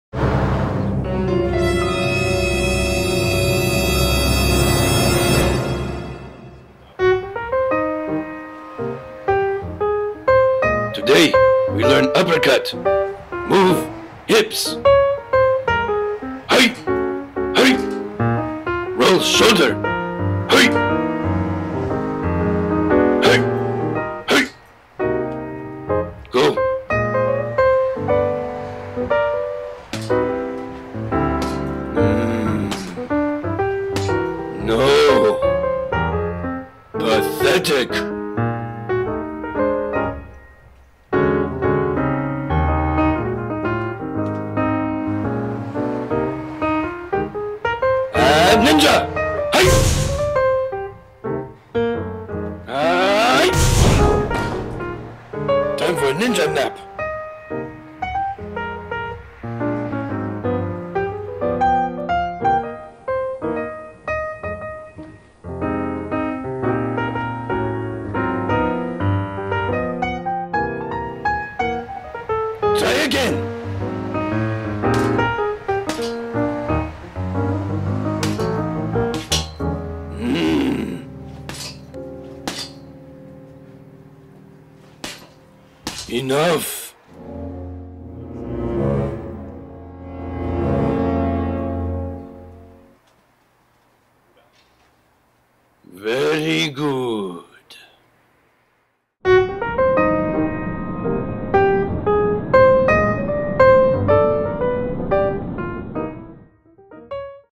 Pro Boxing Ninja Uppercut Training
At Pro Boxing in Pasadena, CA, Michael Milian, the ninja trainer, goes through the finer points of throwing an uppercut with his pupil that is still a little wet behind the ears.